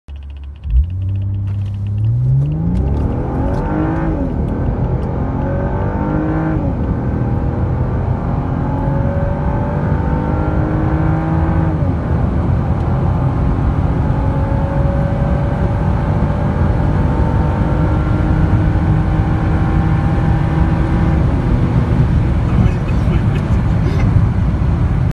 Volvo S60 T6 stage 2 0-200 acceleration (348 hp / 581 Nm)
-down pipe 89”, exhaust fox and borla 76”
-k&n air filter
-mod intercooler
-chip by lab of speed ( Saint-Petersburg)
Best time by race logic 0-100 is 4.9 sec on cold road.